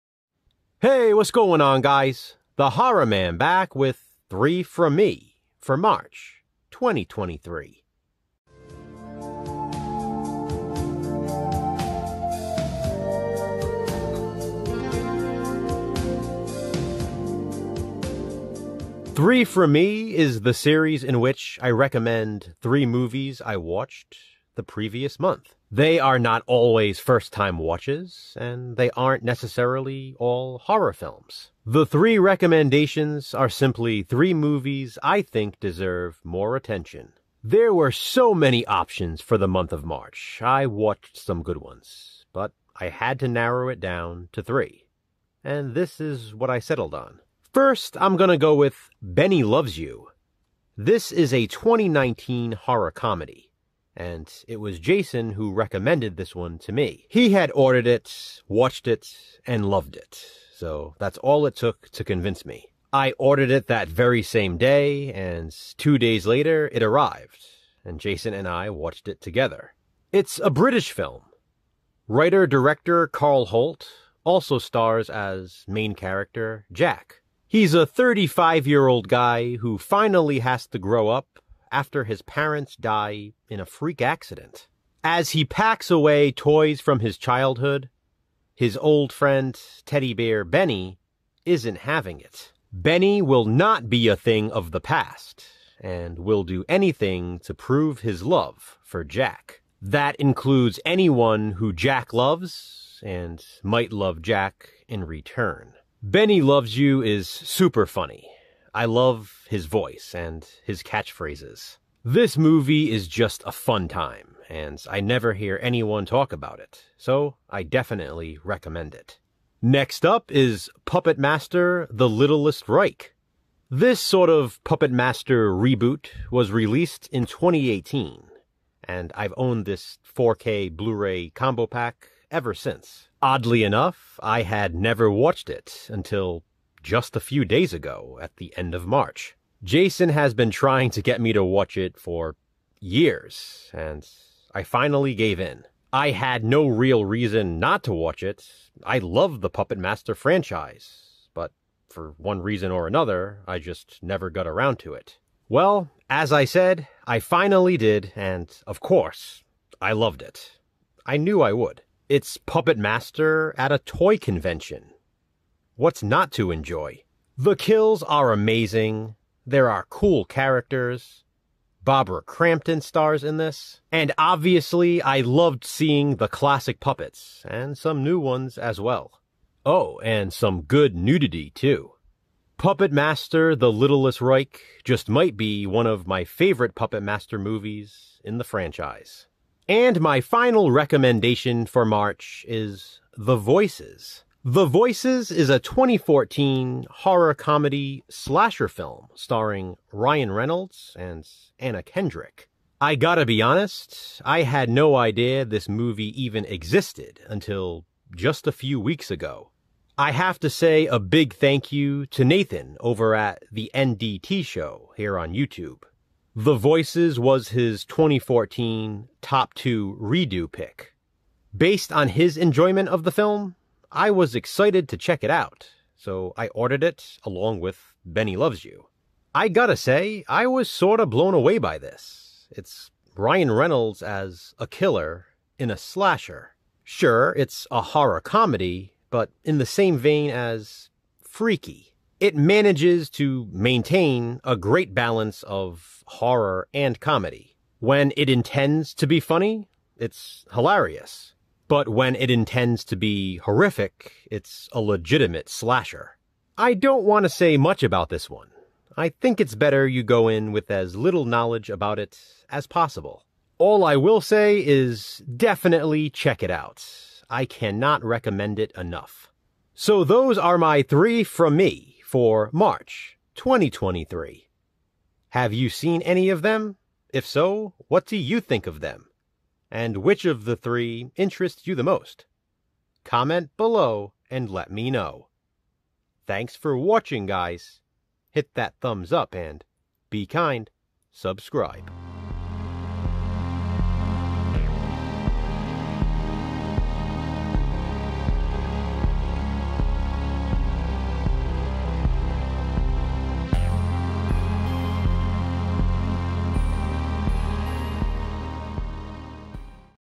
3 FROM ME - MARCH 2023 RECOMMENDATIONS
Highlighting 3 movies I watched last month.